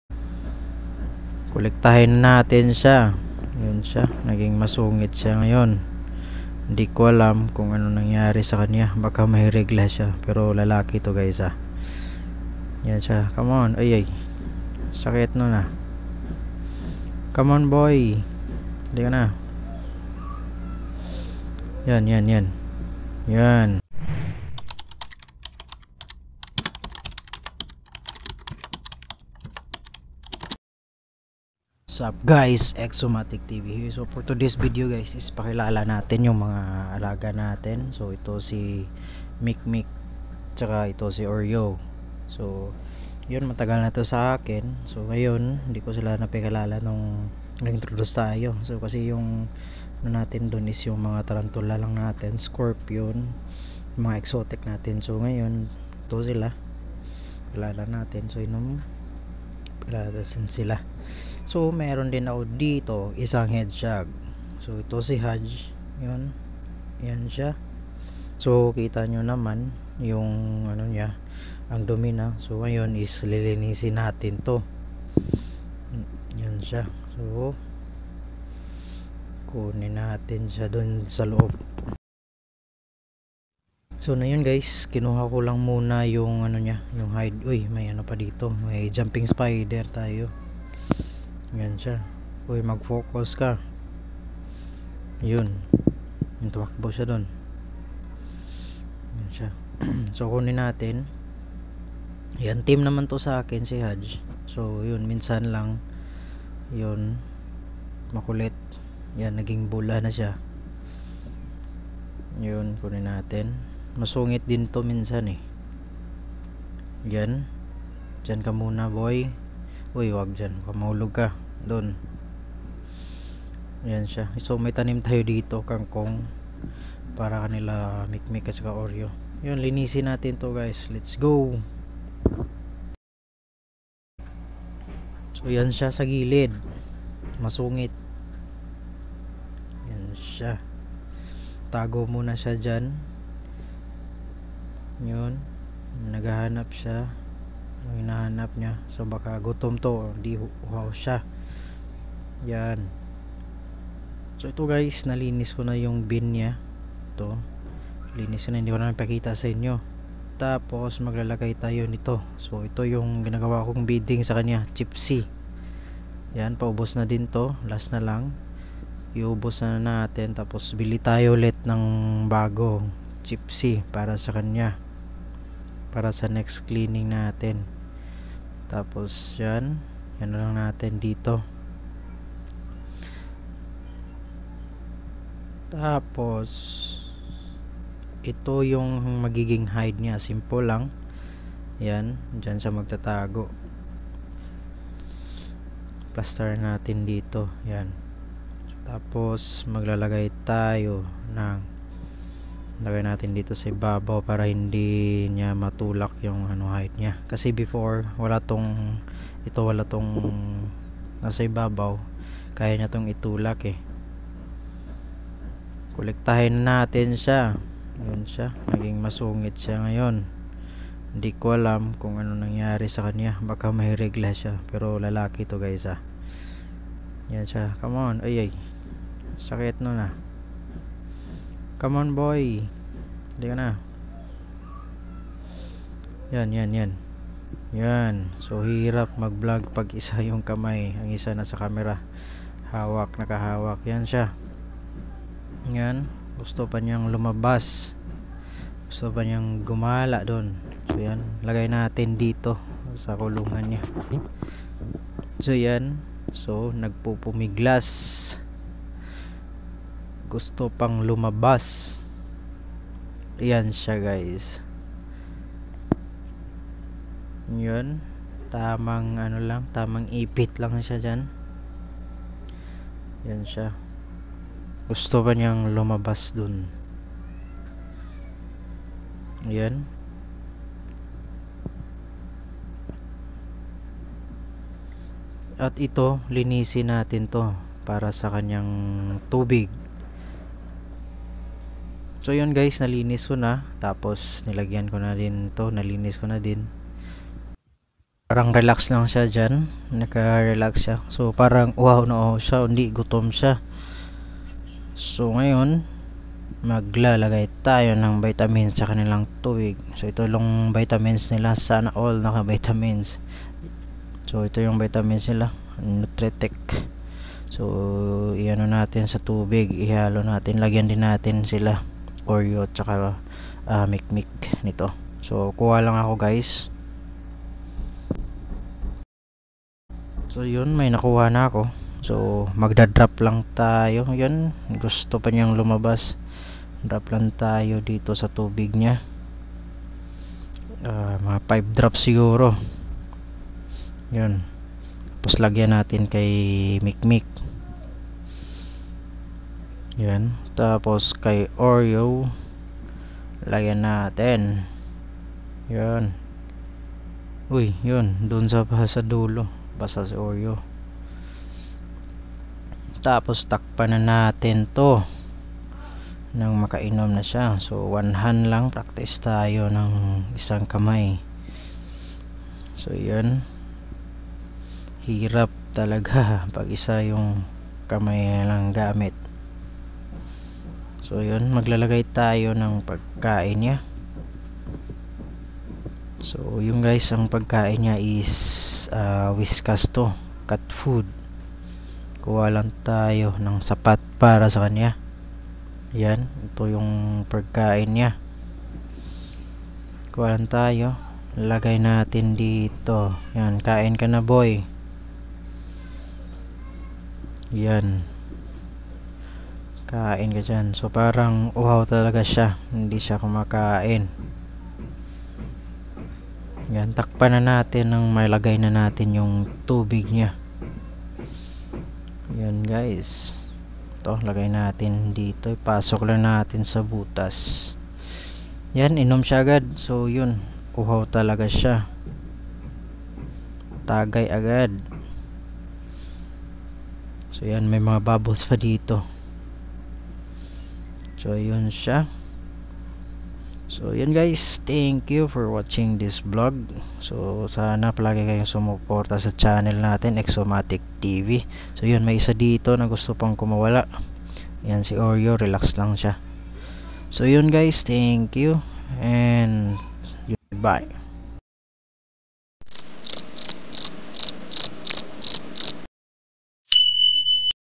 Off muna tayo sa exotics natin. Pakilala muna natin sila.
Matagal na to sa akin sila hodge,oreo and mikmik. Sila ang bagong subaybayan natin sa exomaticTv.